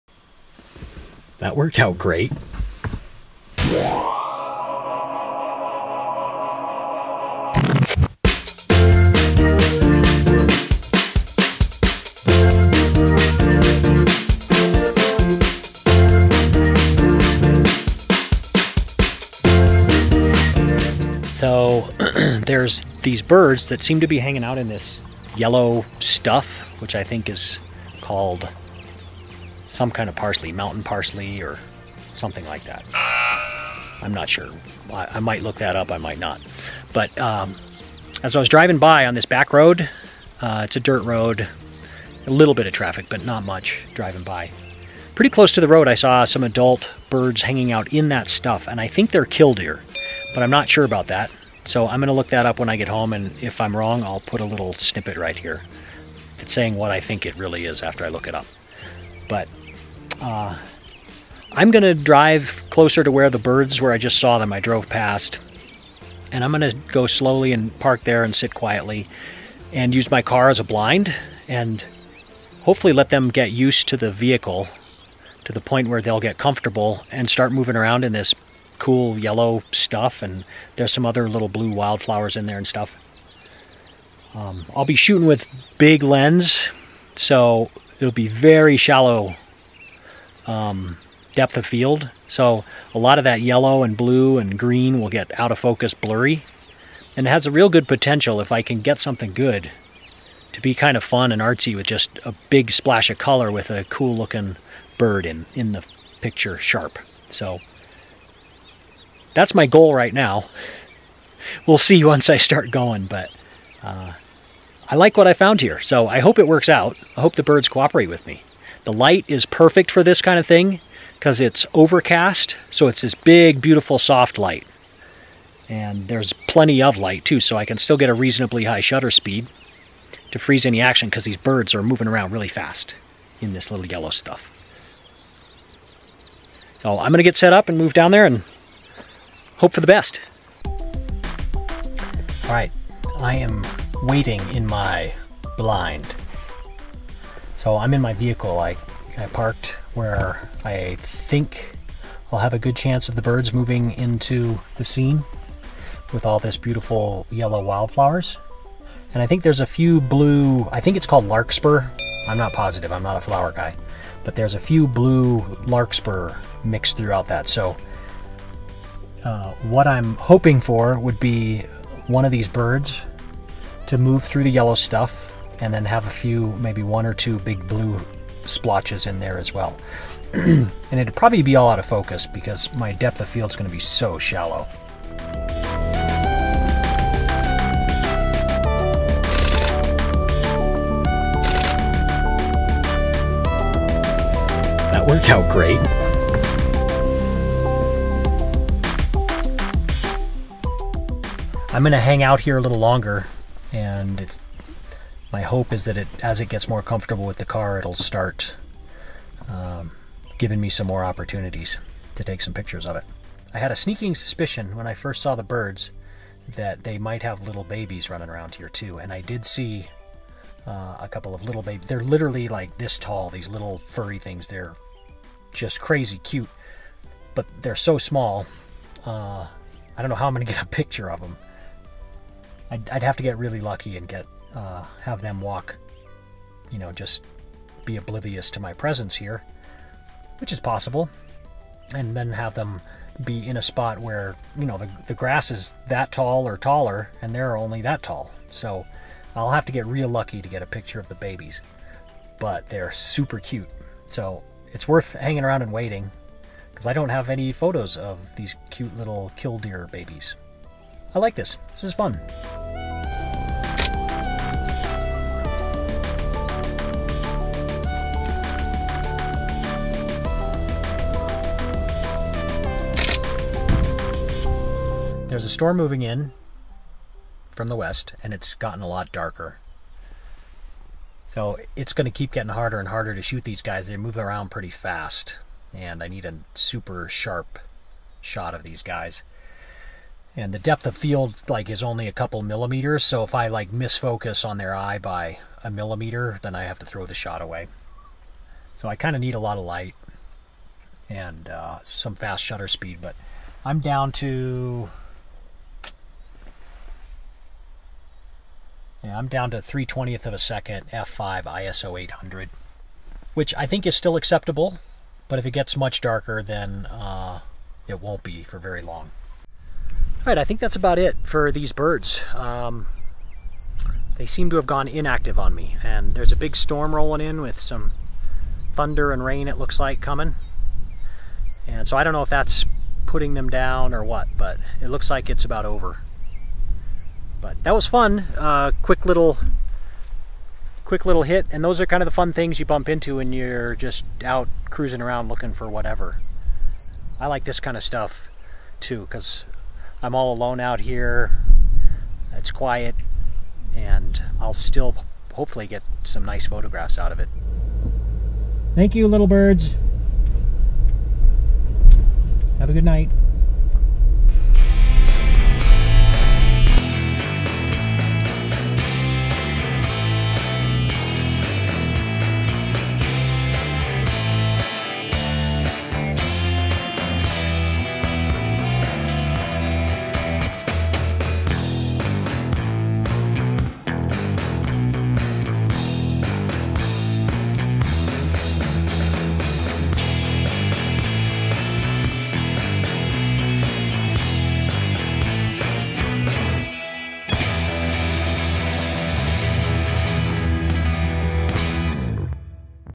Bird Photography | A Killdeer in the Tetons
Join me as I photograph Killdeer birds in the Tetons. A photography vlog.

Don't forget to click 'notifications' so you're alerted when a new video is released.

Thanks,
Steve